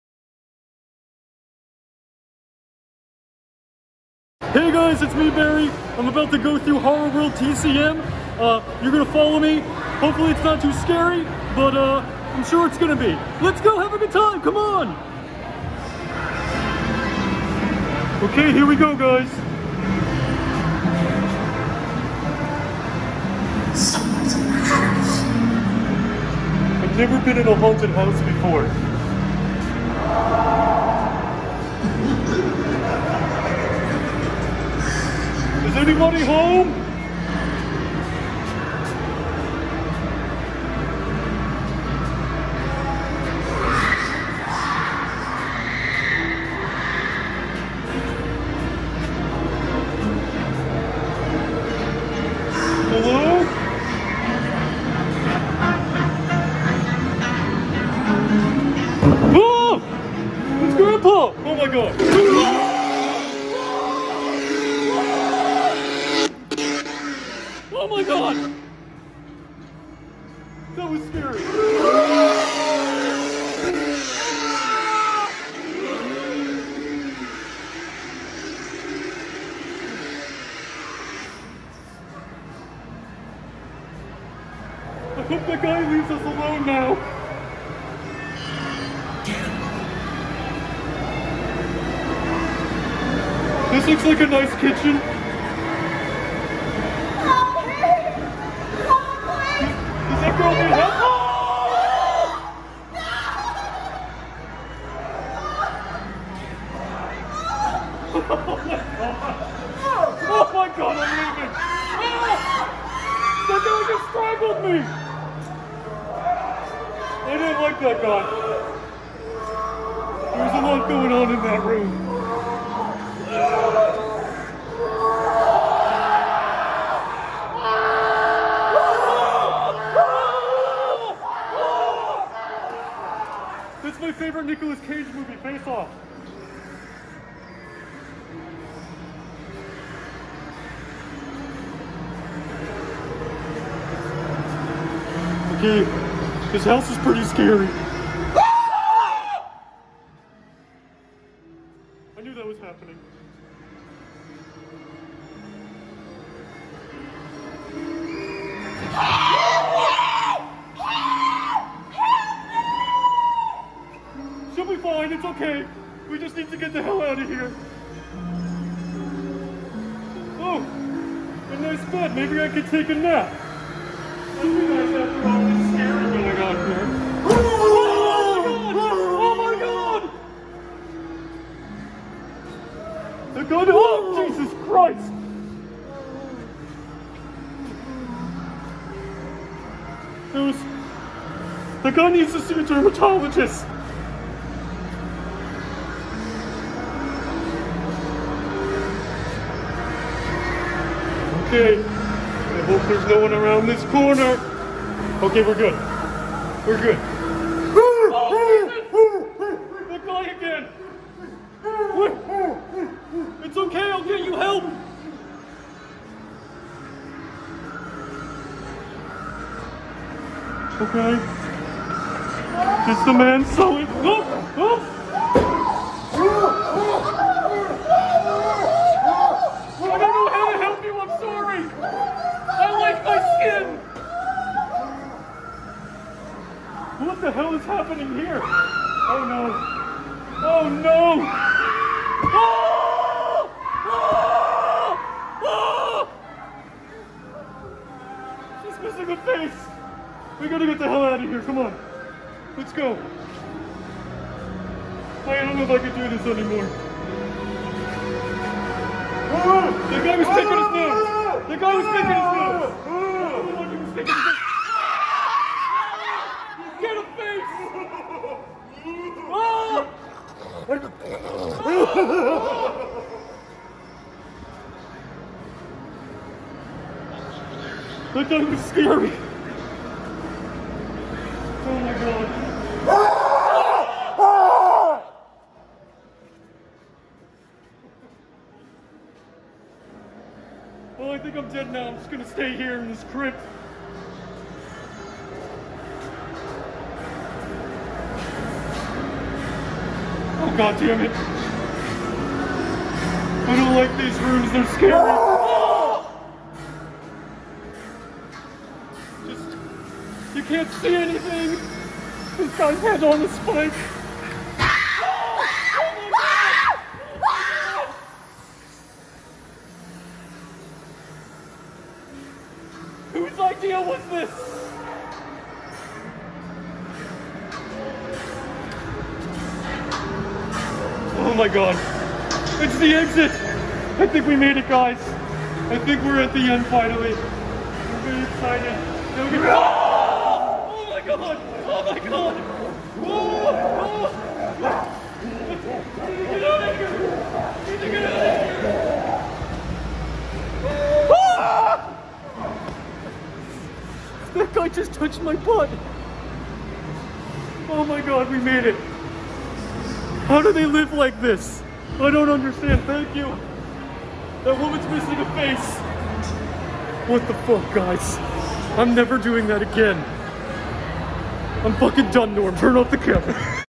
Barry Visits HorrorWorld TCM
Barry Under Your Bed visits HorrorWorld TCM on its closing night to get scared. You would think that the monster living under your bed is used to scary, but just watch what happens.